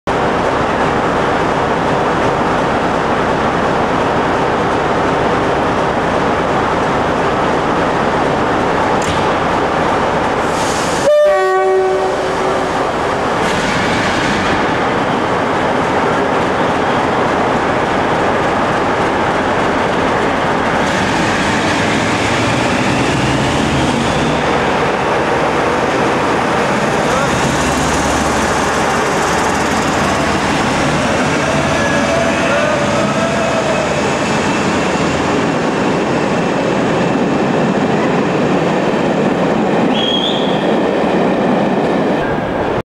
47288 Preston 16.05.92
On the first day of the Summer timetable 1992 no heat 47288 thrashes out of Preston on the SO 2J92 09.00 Barrow Manchester , booked for a no heat Tinsley RFD 47.